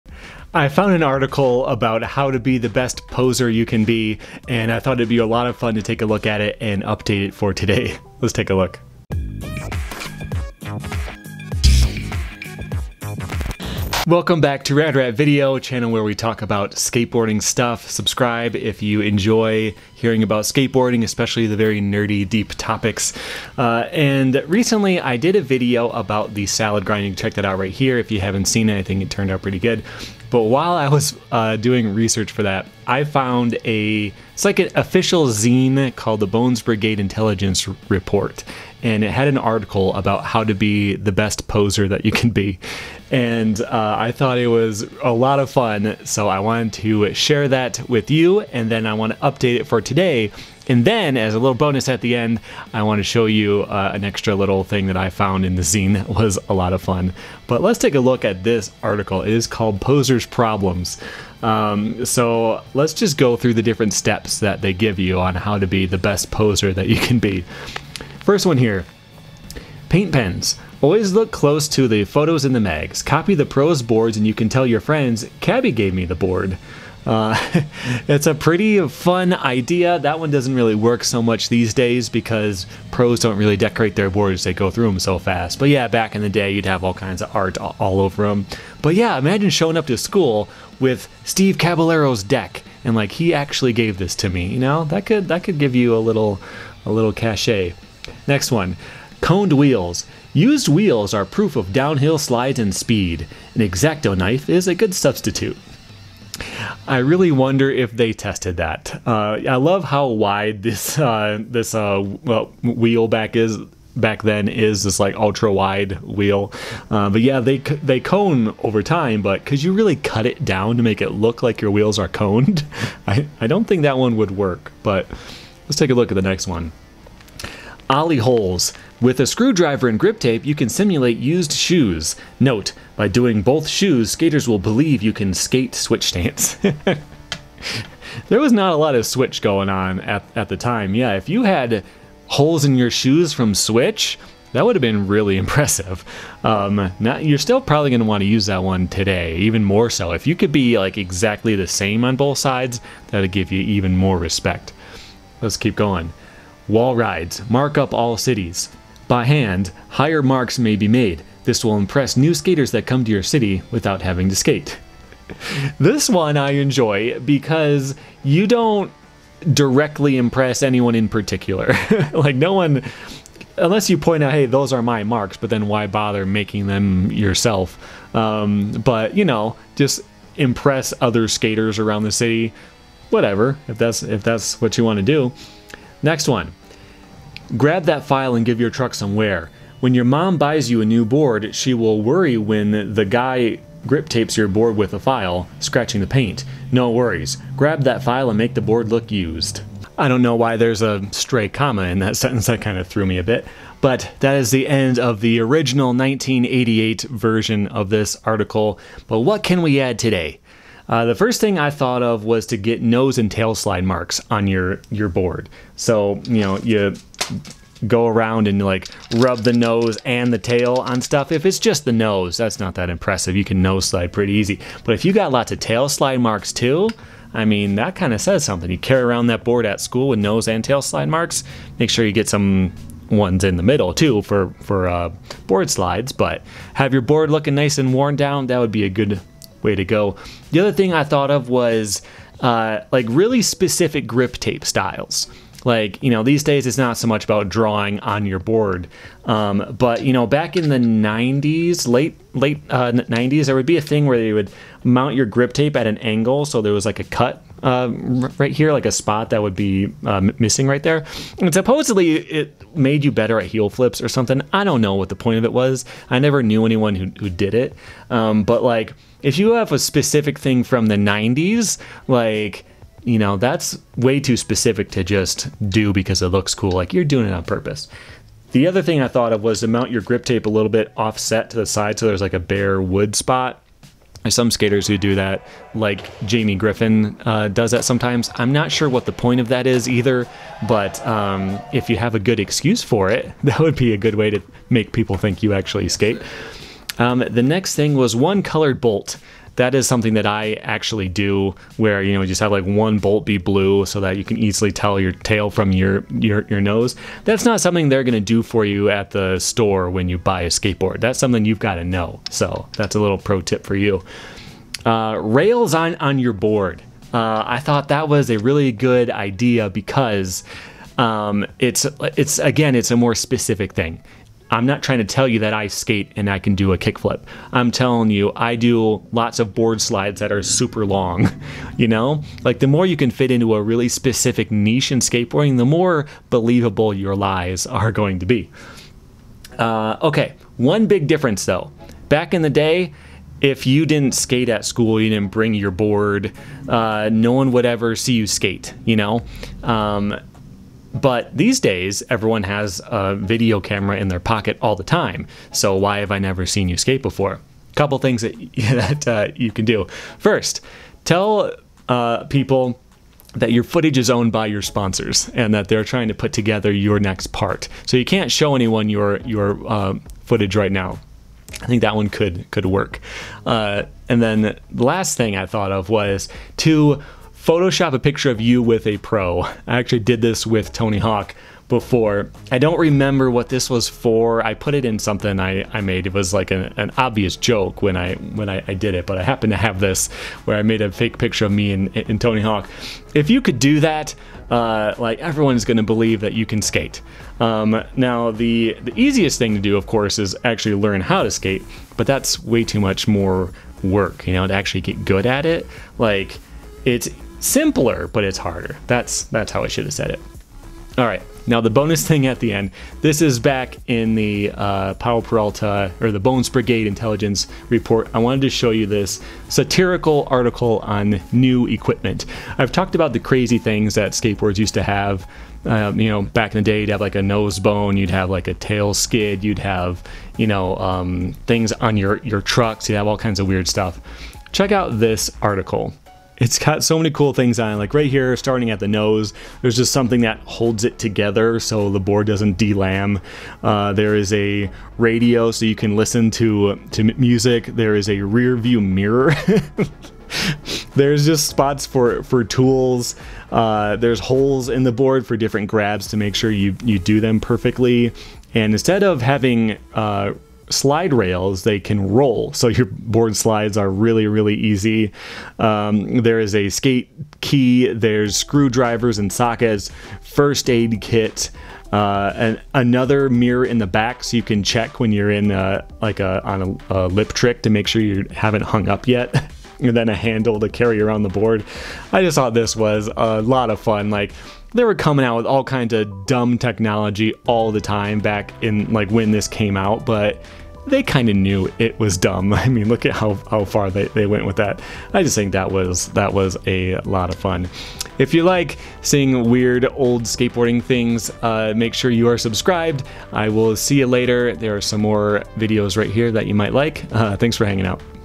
How to Be a Better Poser... According to Powell Peralta (and me)
I found an article in Bones Brigade Intelligence Report from 1988 about how to be a convincing poser. Let's take a look at it, and update it for today! If you want to check out this zine yourself, I found it on archive.org. I might have to check out the rest of the issues for some more of this fun stuff.

0:00-0:13 Intro
0:13-0:58 Bones Brigade Intelligence Report
0:58-3:26 Be a better poser 1988 edition
3:26-6:58 Be a better poser 2023 edition
6:58-8:27 Bonus - new tech cartoon

Shoutout and thanks to channel members: Camille Lepallec, Doug Erberle, paulyk81, KAOTICZ, Lightning, Domicidal, NellyDaHobo